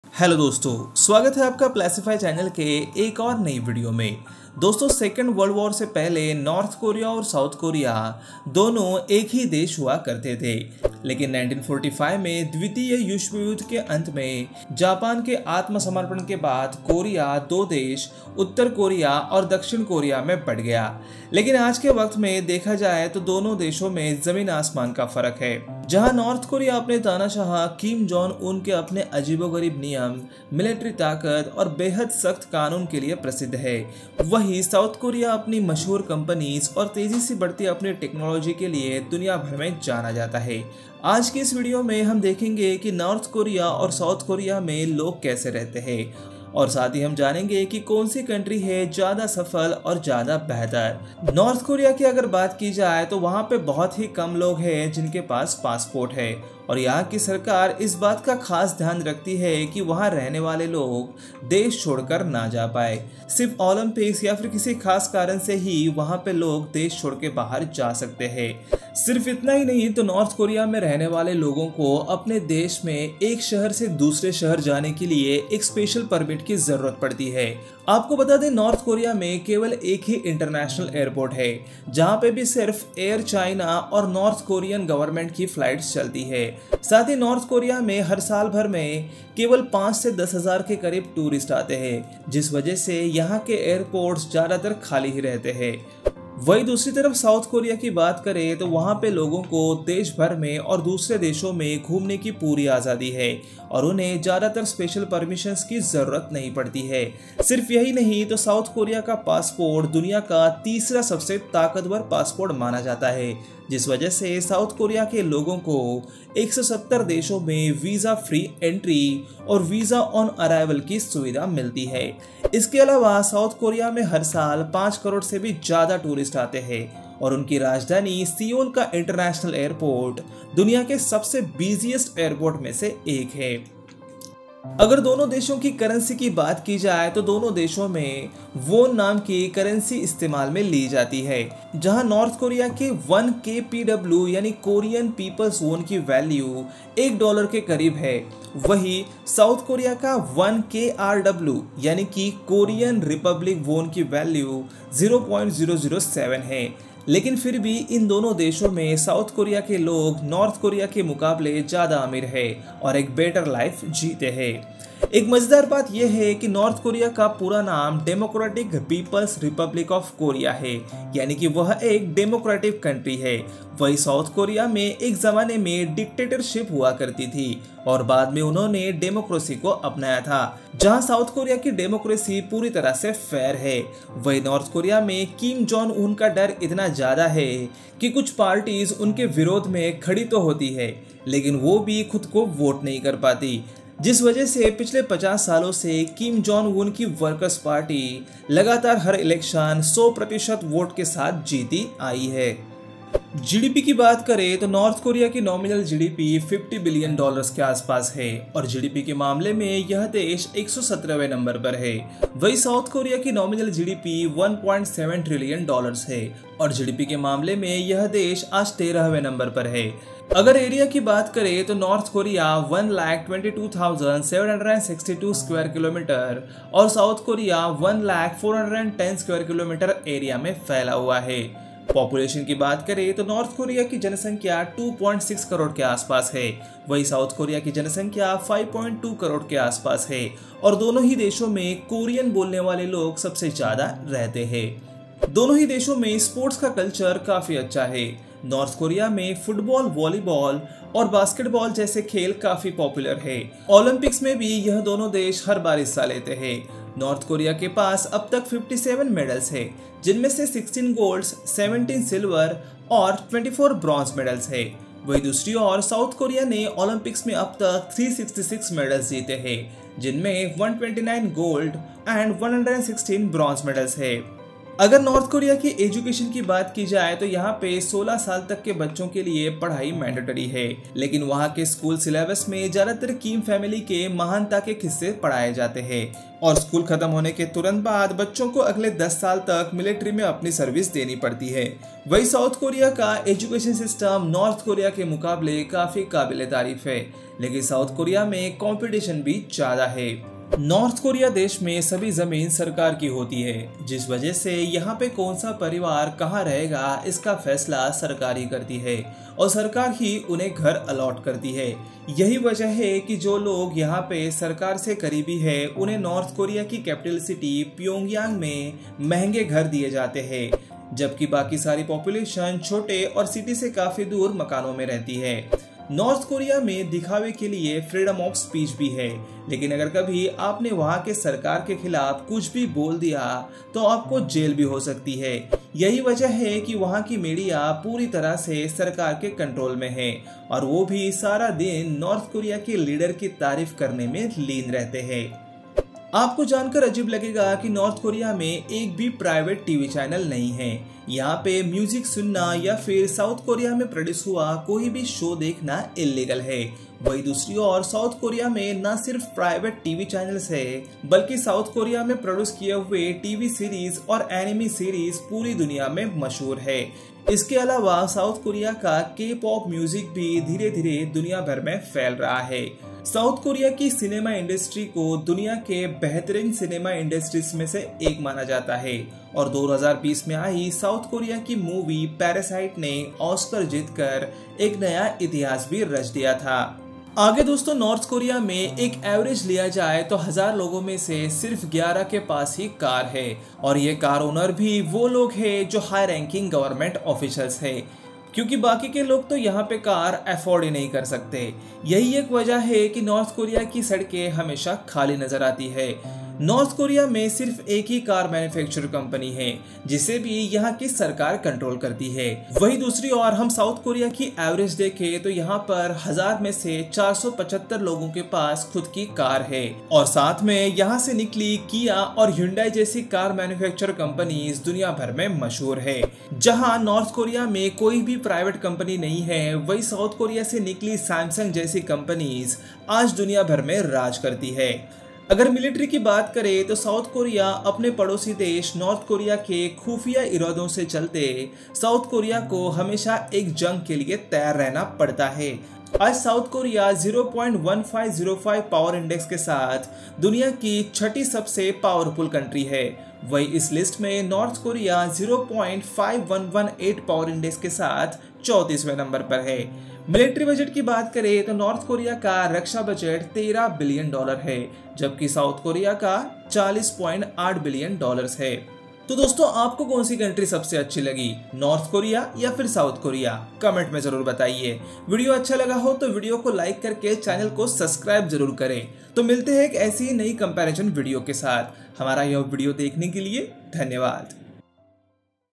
North Korea VS South Korea Country Comparison in Hindi 2024
About: The Korean Peninsula is divided into two countries: North Korea  and South Korea . The division between North and South Korea dates back to the end of World War II in 1945, when Korea was liberated from Japanese occupation. In this video we have compared this two countries on the basic of GDP, Economics, Cost of living and many other factors. Do let le know which country you like the most?

► Watch this video carefully and answer the question asked at the end of the video.

► Mouse & Keyboard